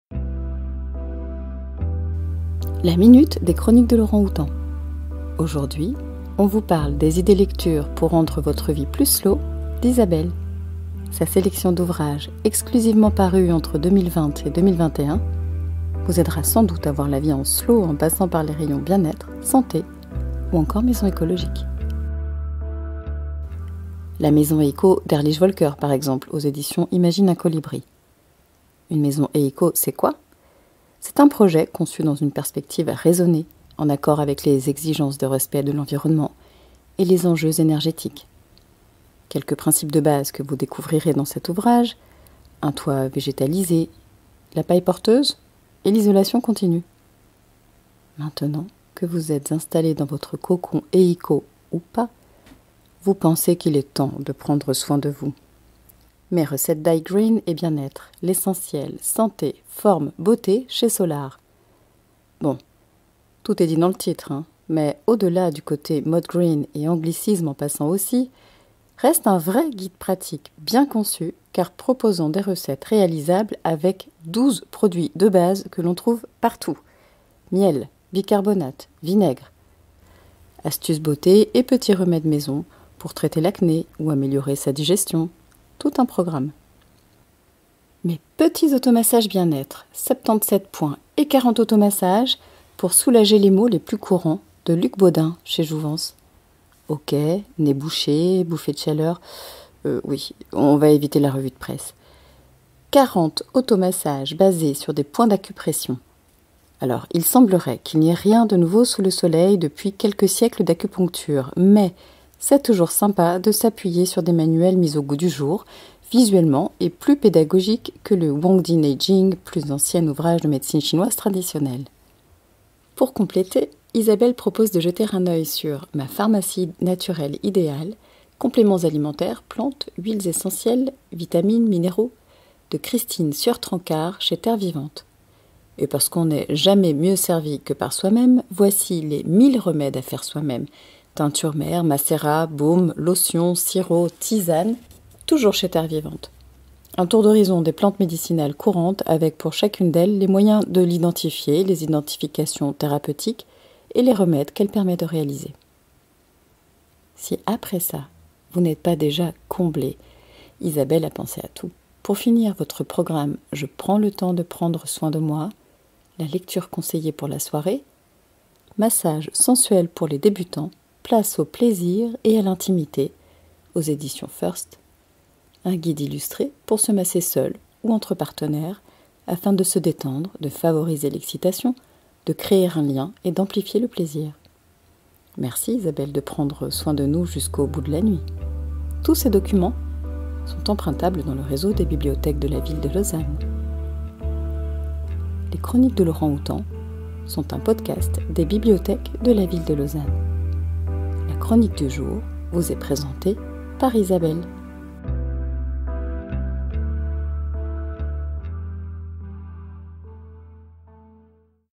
[S L O W] Ralentir et prendre soin de soi | Podcast #49
Et si on ralentissait un peu?
Voici la sélection d'Isabelle d'ouvrages aux rayons santé, bien-être et écologie.

Les œuvres citées:
La maison Heiko, de Volker Ehrlich, Imagine un colibri, 2020
Mes recettes DIY green et bien-être, de Julien Kaibeck, Solar, 2021
Mes petits auto-massages bien-être, de Luc Bodin, Jouvence, 2021
Ma pharmacie naturelle idéale, de Chrisitine Cieur-Tranquard, Terre vivante, 2021
1000 remèdes à faire soi-même, de Claudine Luu, Terre vivante, 2021
Massage sensuel pour les débutants, de Sydney Price, First Editions, 2021

et prendre soin de soi